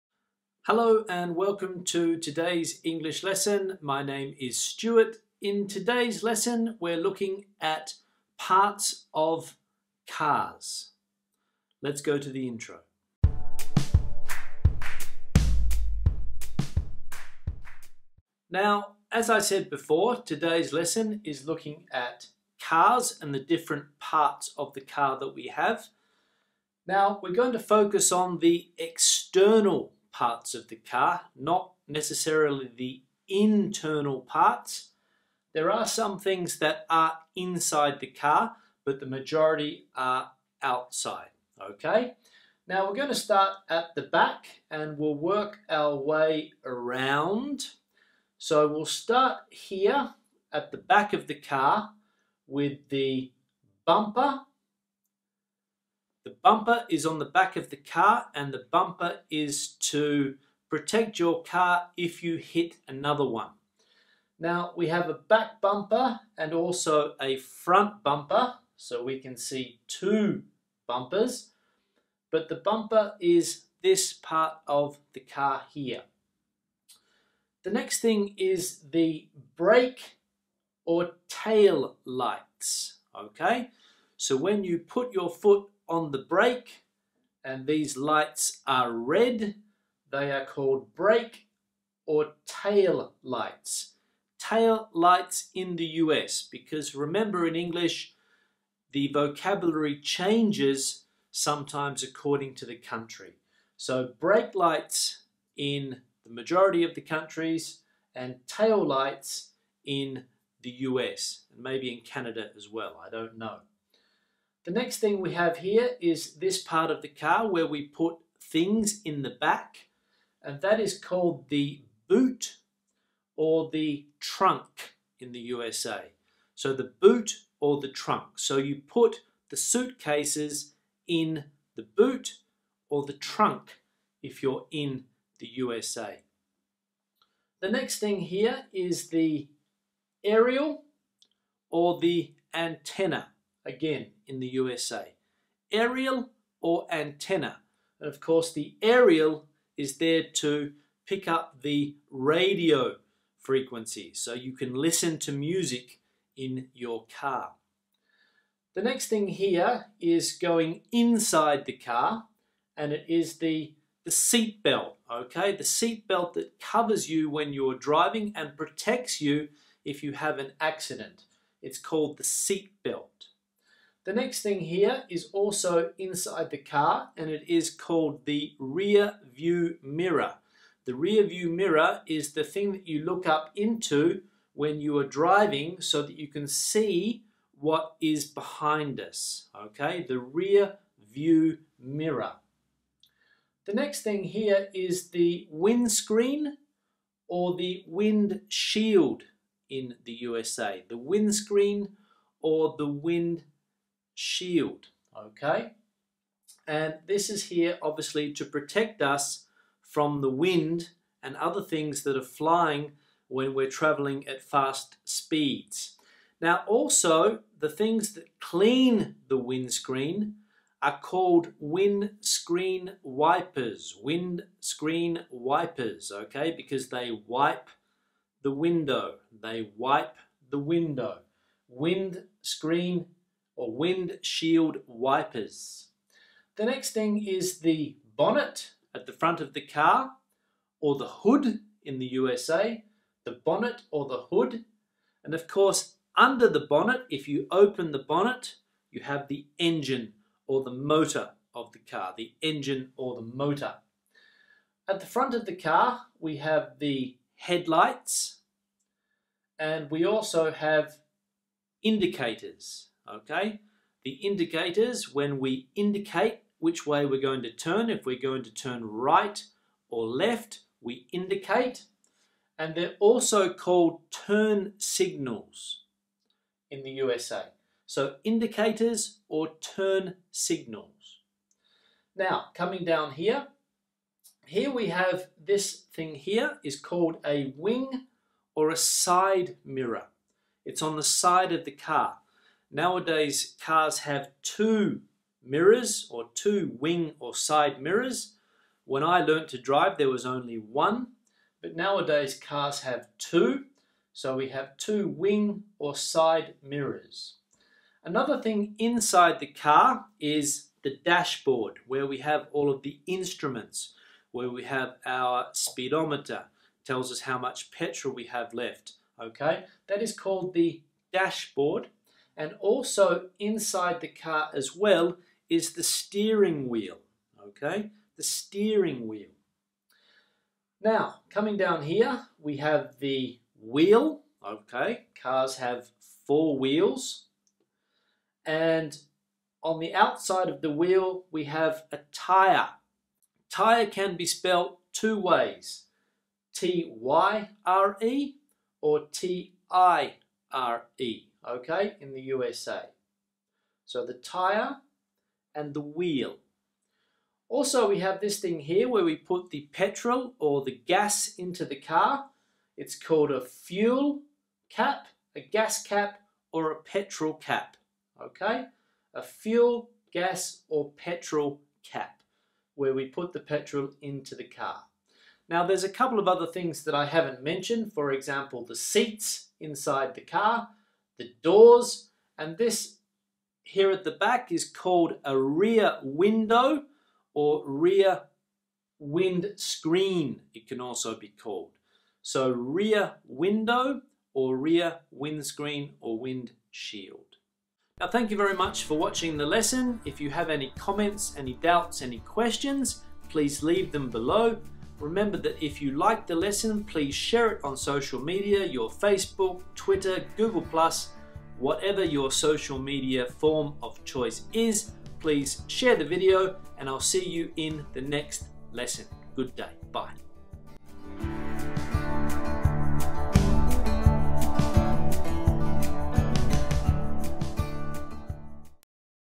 English vocabulary lesson A2 - Parts of a car - Vocabulario en inglés
Improve your vocabulary in English with this lesson on the words we use to talk about cars, like boot, windscreen and wheel.  This lesson is for students at the A2 or pre-intermediate level.